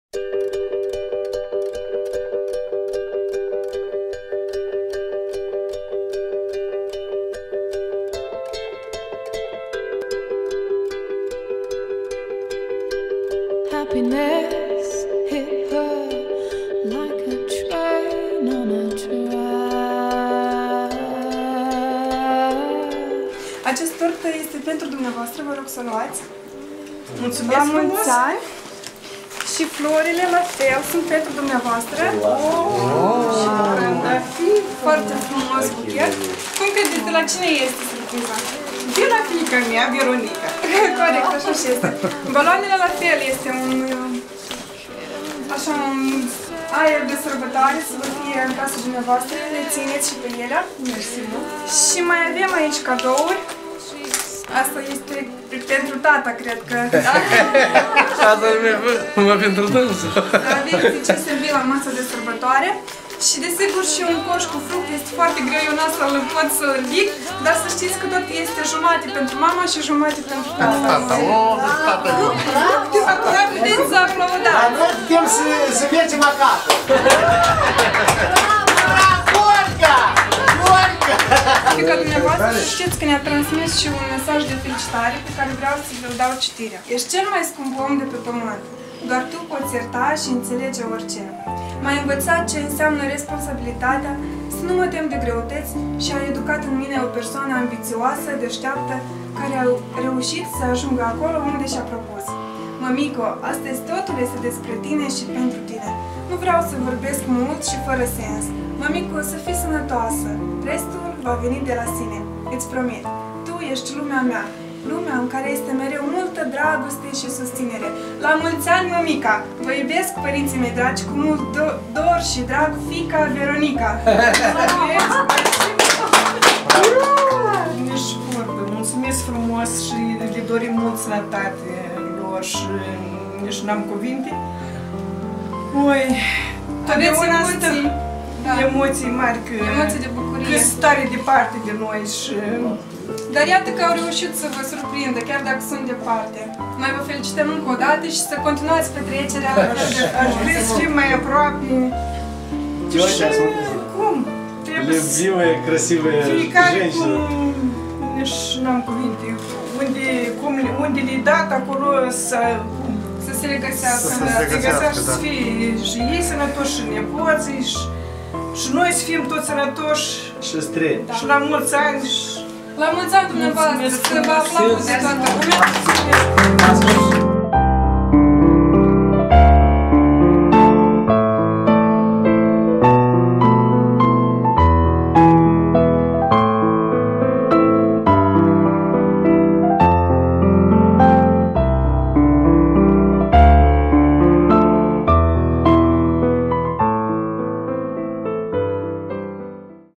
Surprize Surprize - Livrare Flori sat. Talmaza
O Surpriza frumoasa pentru Doamna Maria Omagiata, Si Sotul ei Nicolae.
  surpriza provine din Partea ficei sale Veronica.